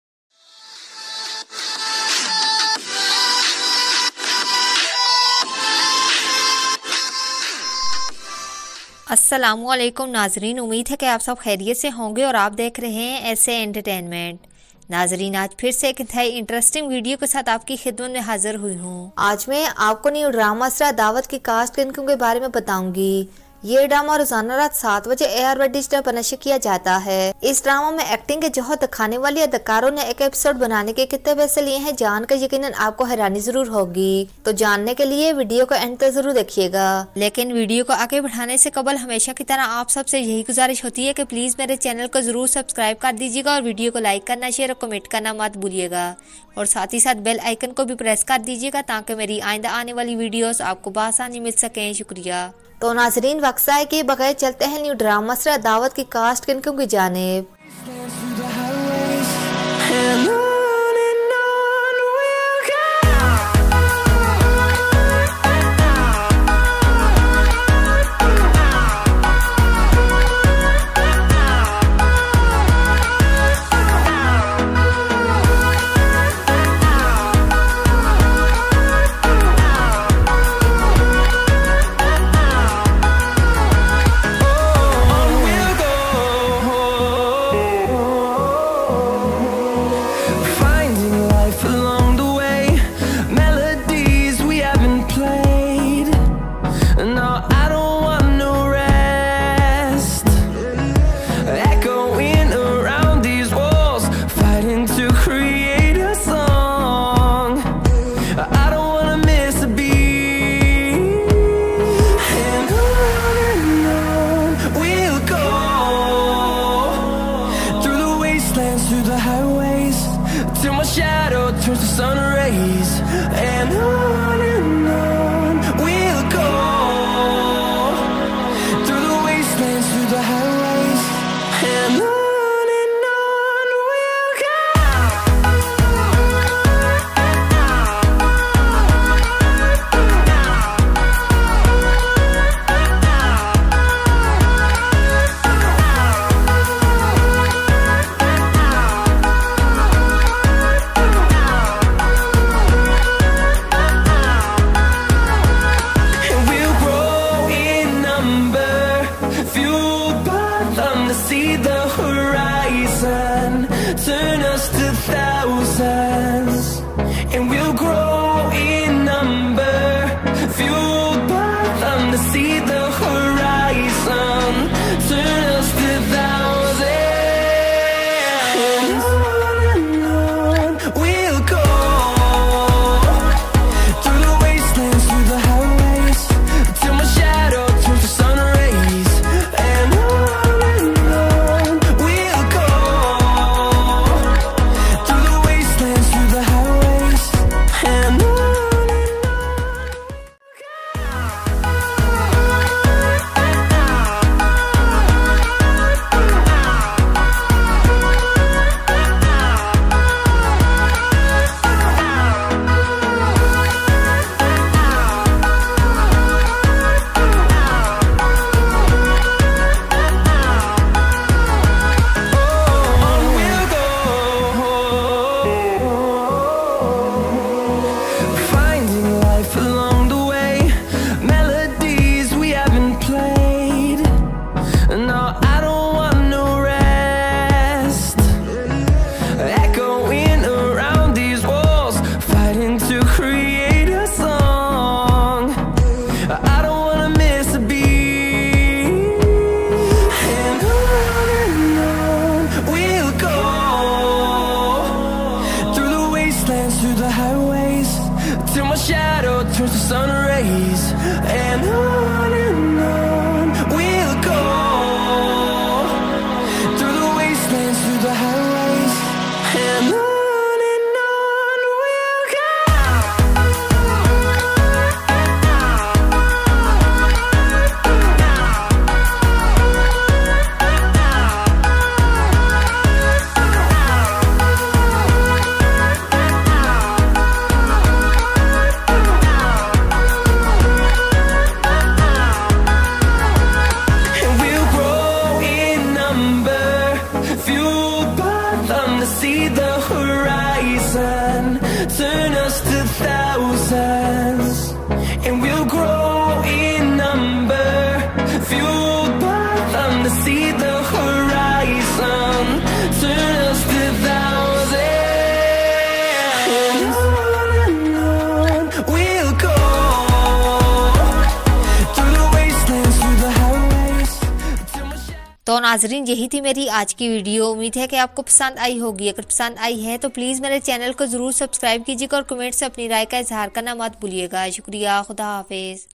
Adawat Drama Cast Salary Last Episode 63
Assalamualaikum
Adawat drama cast salary. New ary digital drama all cast real names. Fatima effendi syed jibran and saad qureshi new episode adawat starting from 12 December every Monday to Sunday tonight 7 00 pm only on ary digital.